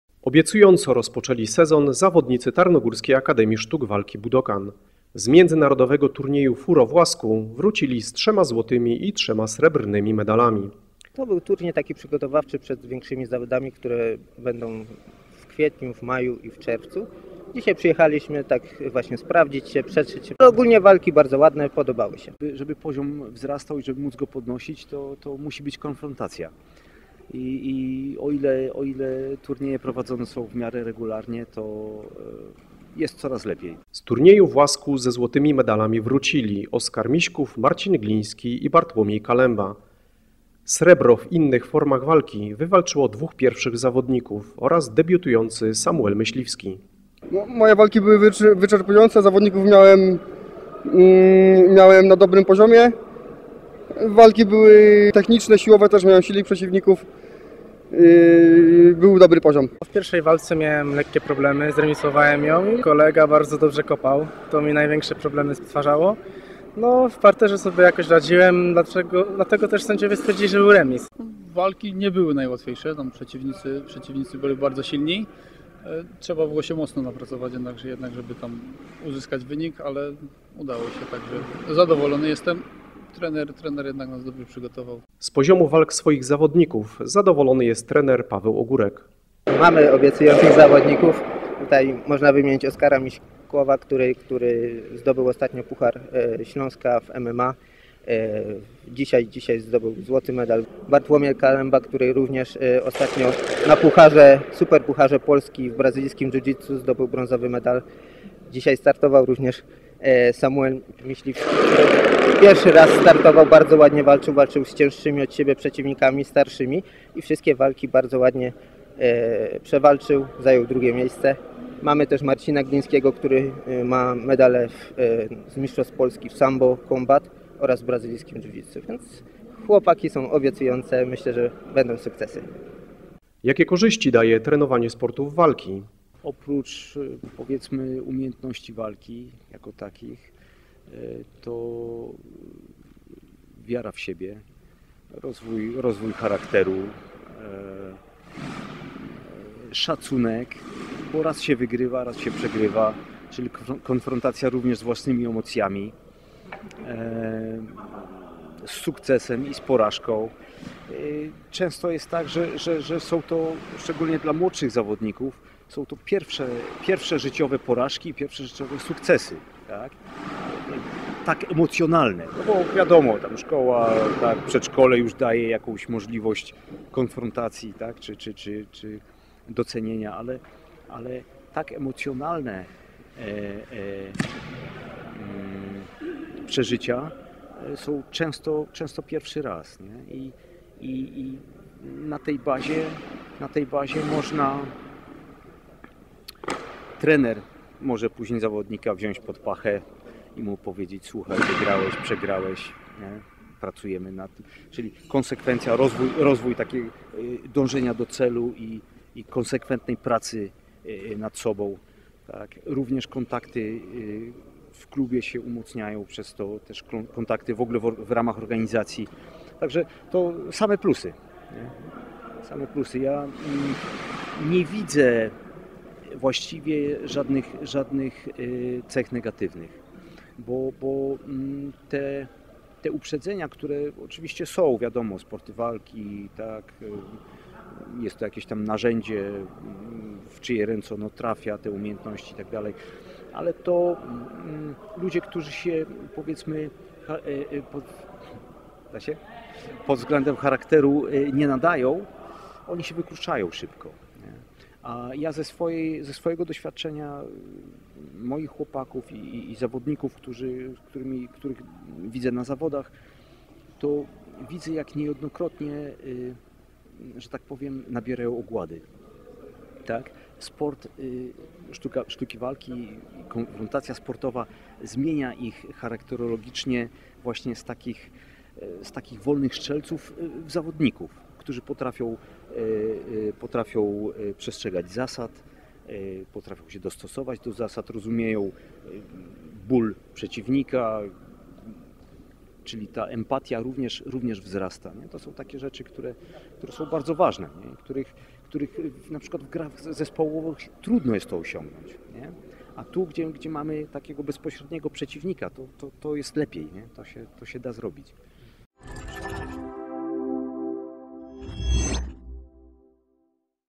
Budokan Tarnowskie Góry - medale z Łasku
Obiecująco rozpoczęli sezon zawodnicy Tarnogórskiej Akademii Sztuk Walki Budokan. Z Międzynarodowego Turnieju Furo w Łasku wrócili z 3 złotymi i 3 srebrnymi medalami.